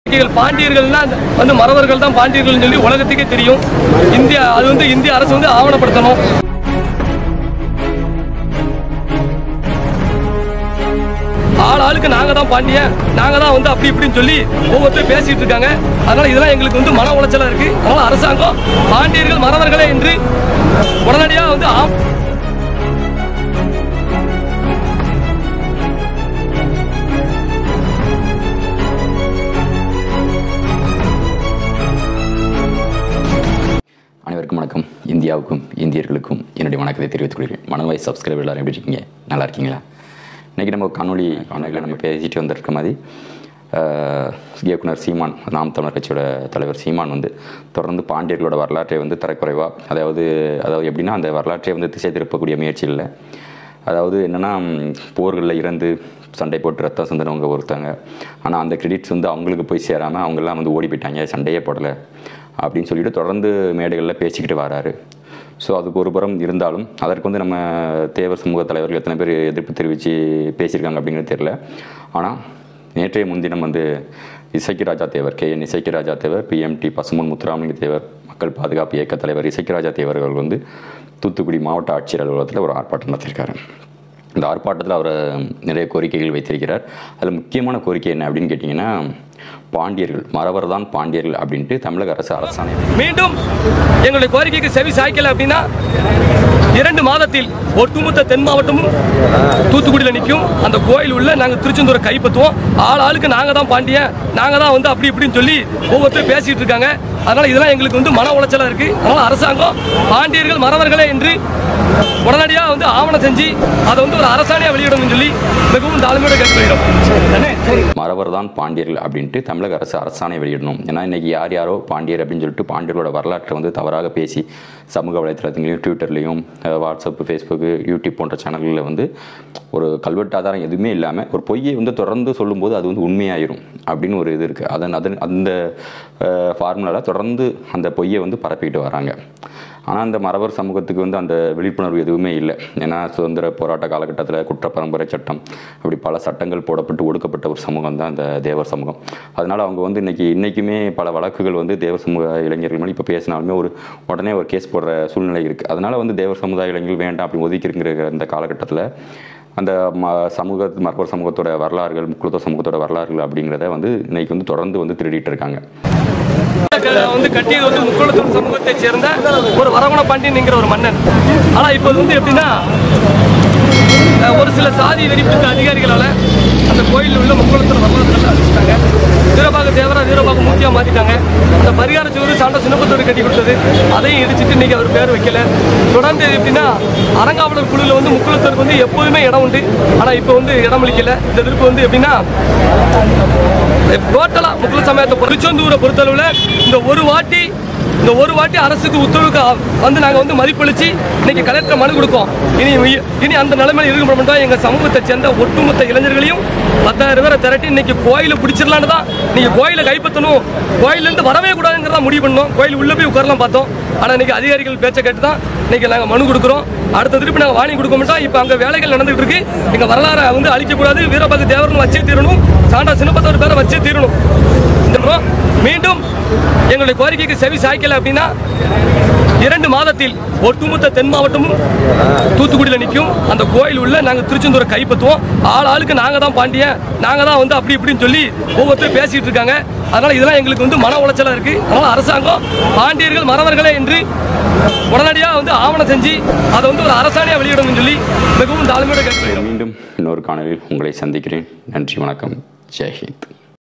"மறவர்களே பாண்டியர்கள்'- அரசாணை வெளியிட கோரி K.N.இசக்கிராஜாதேவர் ஆர்ப்பாட்டம்
பாண்டியர் வரலாறு மீட்பு ஆர்ப்பாட்டம் இசக்கிராஜாதேவர் ஆவேசம்